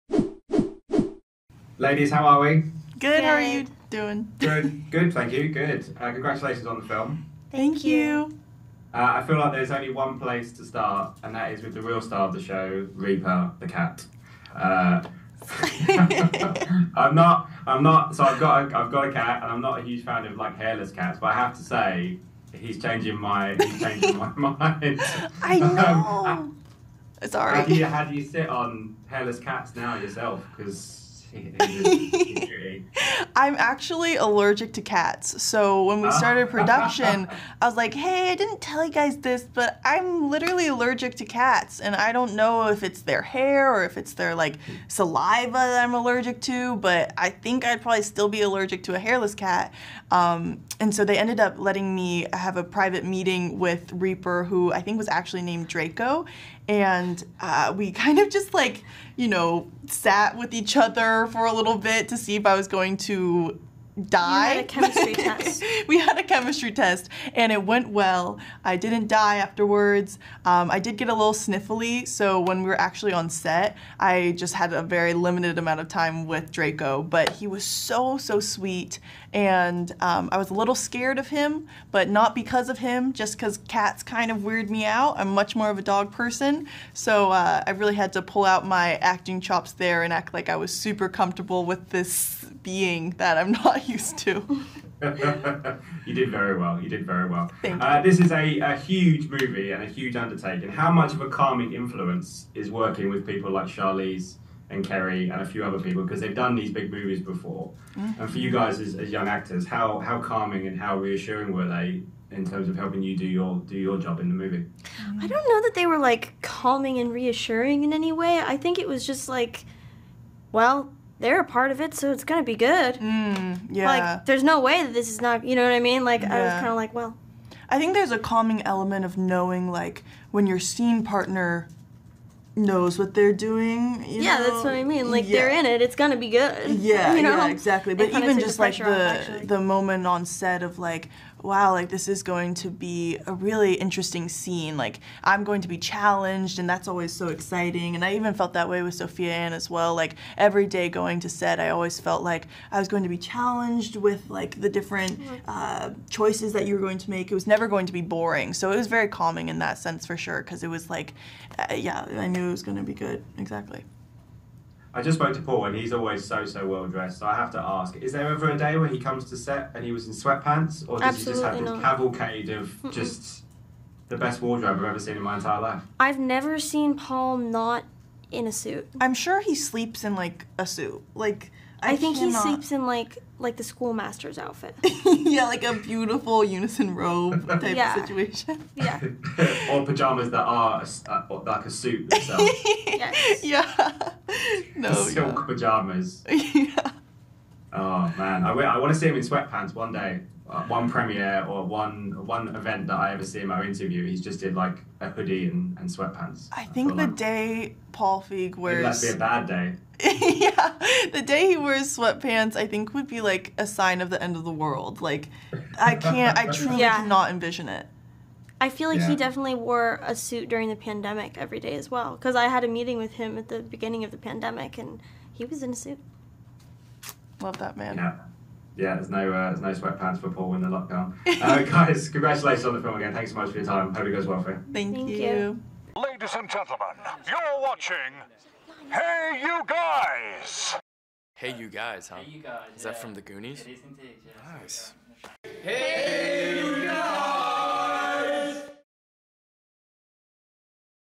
The School of Good and Evil - Sophia Anne Caruso & Sofia Wylie on Reaper the cat, Paul Feig & more
Sophia Anne Caruso & Sofia Wylie are interviewed for Netflix's The School for Good & Evil. Directed by Paul Feig, it stars Sophia Anne Caruso, Sofia Wylie, Laurence Fishburne, Michelle Yeoh, Jamie Flatters, Kit Young, Peter Serafinowicz, Rob Delaney, Mark Heap, Patti LuPone and Rachel Bloom, with Kerry Washington and Charlize Theron. Scott Davis asks the questions.

Plot: In the village of Gavaldon, two misfits and best friends, Sophie (Sophia Anne Caruso) and Agatha (Sofia Wylie), share the unlikeliest of bonds. Sophie, a golden-haired seamstress, dreams of escaping her dreary life to become a princess, while Agatha, with her grim aesthetic and offbeat mother, has the makings of a real witch. One night under a blood red moon, a powerful force sweeps them away to the School for Good and Evil — where the true stories behind every great fairy tale begin. Yet something is amiss from the start: Sophie is dropped into the School for Evil, run by the glamourous and acid-tongued Lady Lesso (Charlize Theron), and Agatha in the School for Good, overseen by the sunny and kind Professor Dovey (Kerry Washington). As if navigating classes with the offspring of the Wicked Witch (Freya Parks), Captain Hook (Earl Cave), and King Arthur (Jamie Flatters) wasn’t hard enough, according to the Schoolmaster (Laurence Fishburne), only true love’s kiss can change the rules and send the girls to their rightful schools and destiny. But when a dark and dangerous figure (Kit Young) with mysterious ties to Sophie reemerges and threatens to destroy the school and the world beyond entirely — the only way to a happy ending is to survive their real life fairytale first.

The School for Good & Evil is out now.